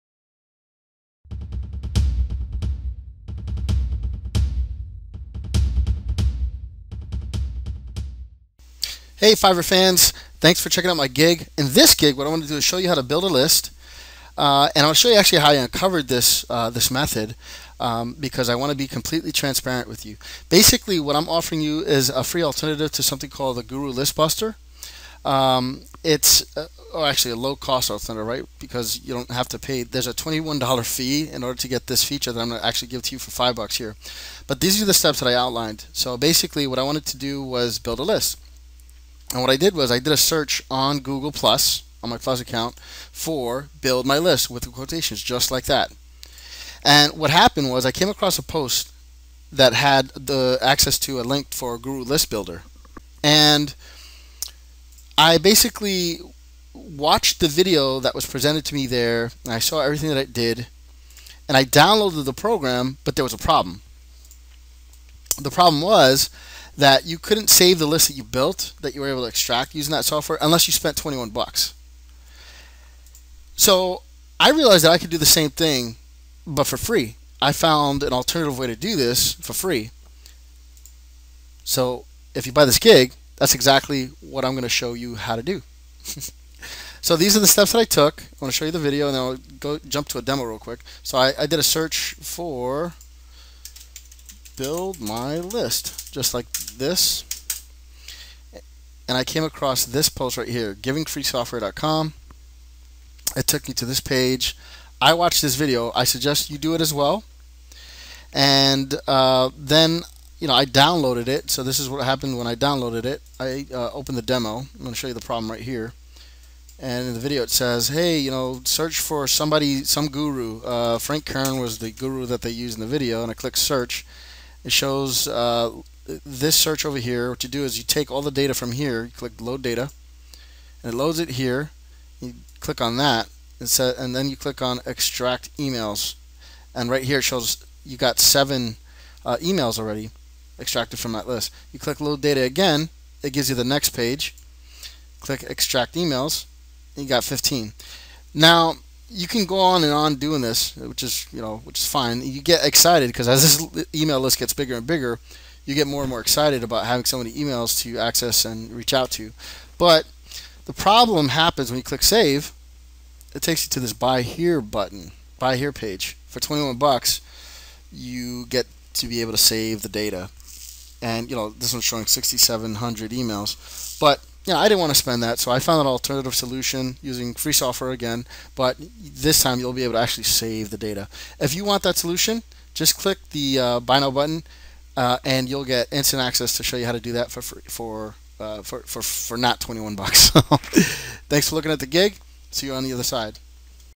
Guru List Builder Alternative - How To Steal Email Lists From Any Company Or Guru Fast
to learn how to instantly build a list for $5.

This gig offers a free software solution alternative to the Guru List Builder software that costs $21 to upgrade and actually save the data.

I was astonished that I was able to find these tools and that I was able to build a list instantly using this technique.

If you're promoting anything and you want to build a massive list very quickly then I highly suggest you take advantage of this offer while its available.